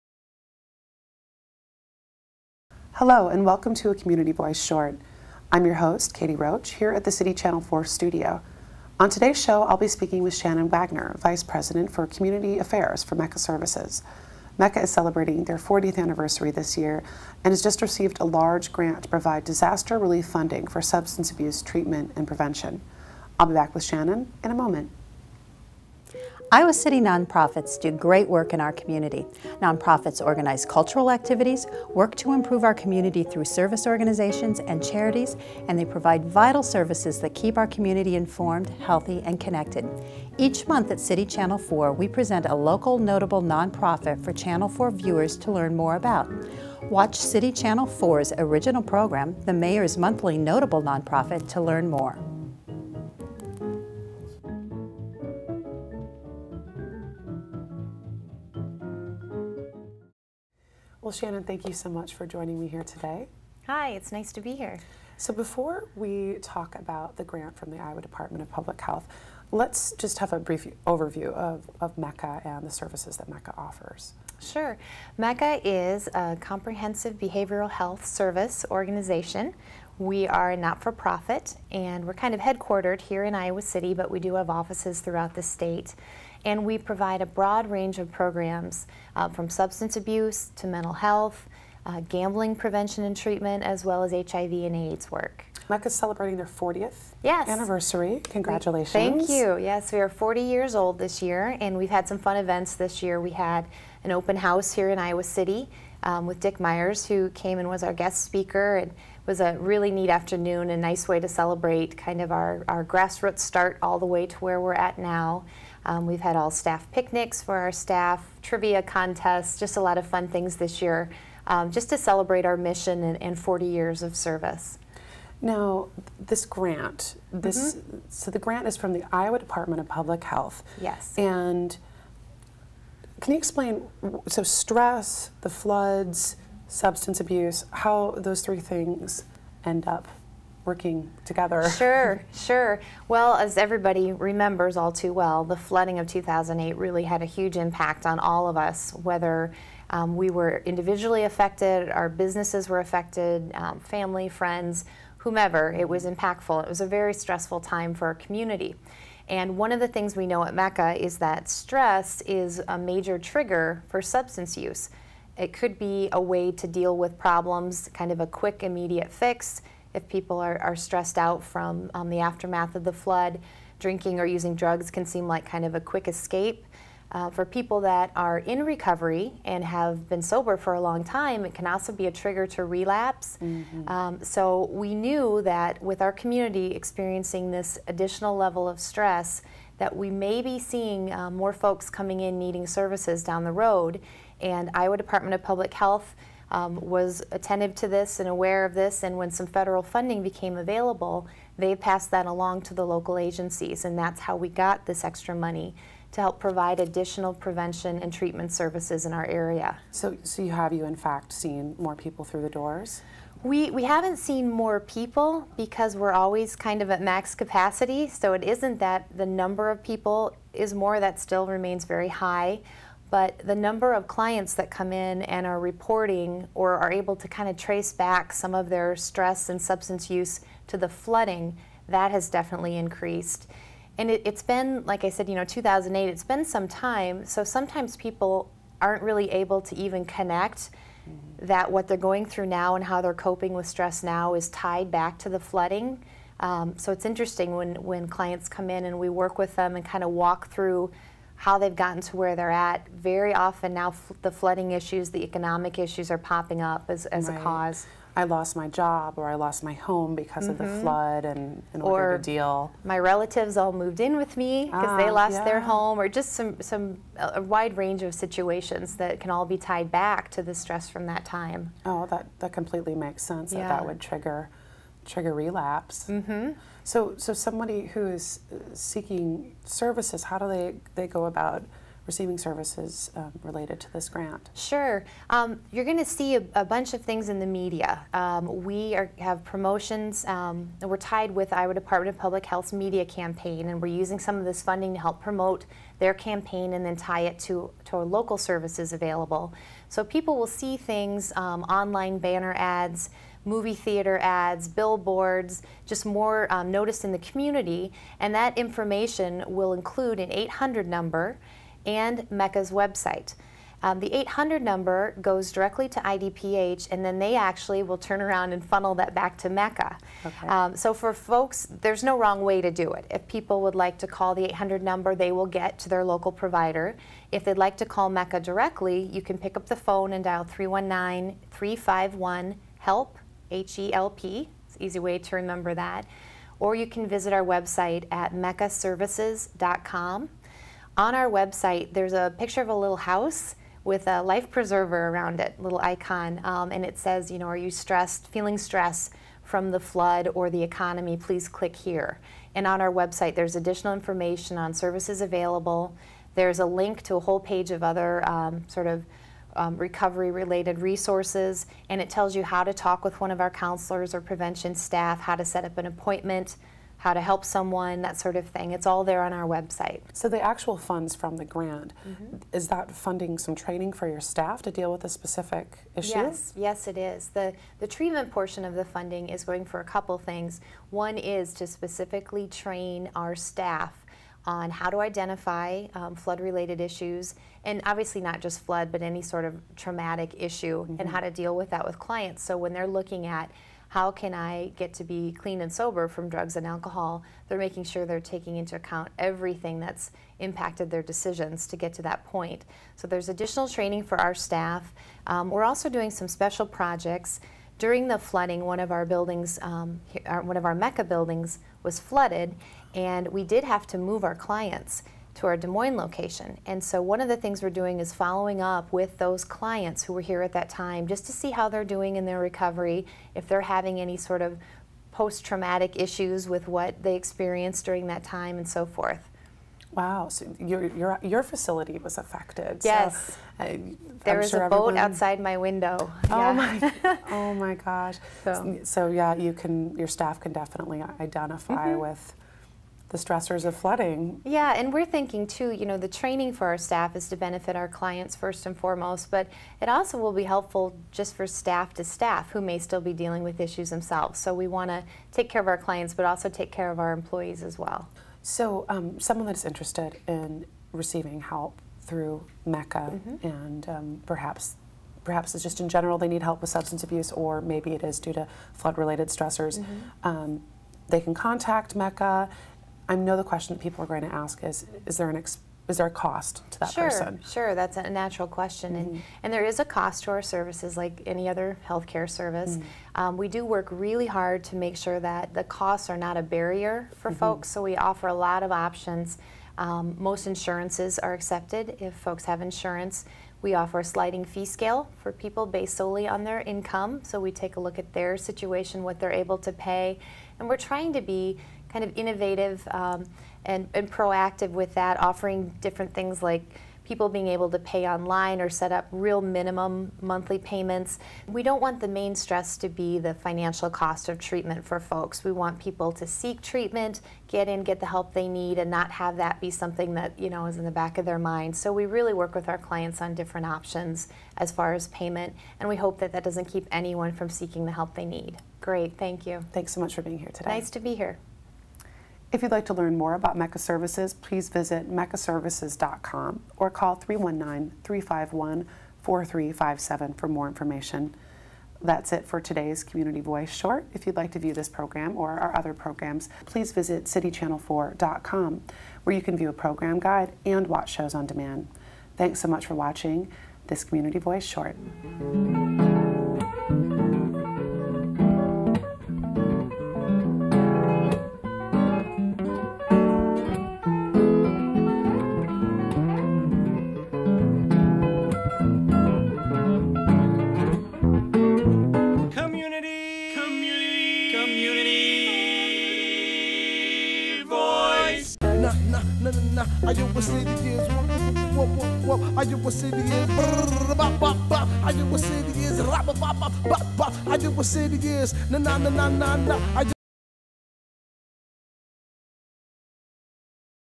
MECCA Services Recovery Grant presented by the Community Voice
Community Voice Host Katie Roche interviews Shannon Wagner, Vice President of Community Affairs for MECCA Services about the grant that MECCA received from the Iowa Department of Public Health to assist people experiencing relapse due to flood related events of the summer of 2008.